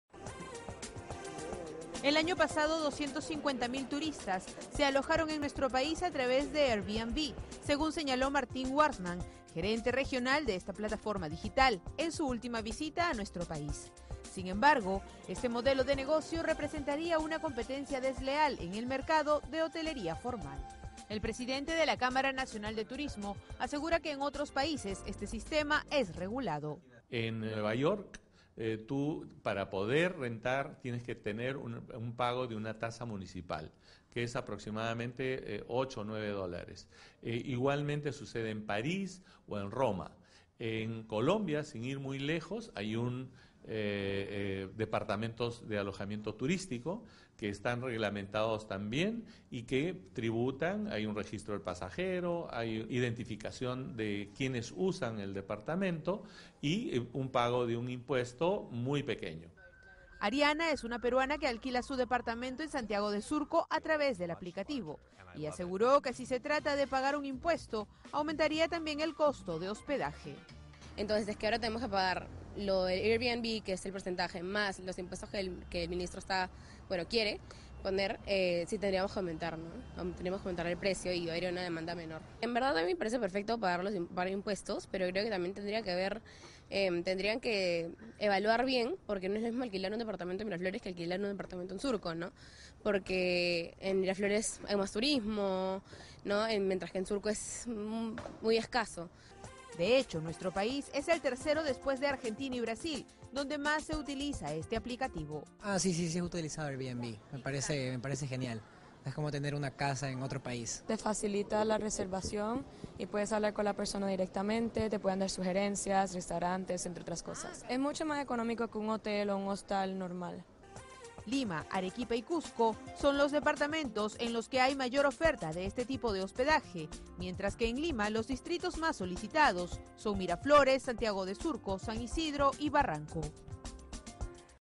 Evalúan regular aplicaciones de alquiler de viviendas
El año pasado 250 mil turistas se alojaron en nuestro país a través de Airbnb, según señaló Martin Warsman, gerente regional de esta plataforma digital, en su última visita a nuestro país. Sin embargo, este modelo de negocio representaría una competencia desleal en el mercado de hotelería formal.

Quienes utilizan esta aplicación para alquilar sus inmuebles han asegurado que, si se trata de pagar un impuesto, aumentaría también el costo de hospedaje. Carlos Canales, presidente de la Cámara Nacional de Turismo, asegura que en otros países este sistema es regulado.

El Perú es el tercer país después de Argentina y Brasil donde más se utiliza este aplicativo. Lima, Arequipa y Cusco son los departamentos en los que hay mayor oferta de este tipo de hospedaje. En Lima los distritos más solicitados son Miraflores, Santiago de Surco, San Isidro y Barranco.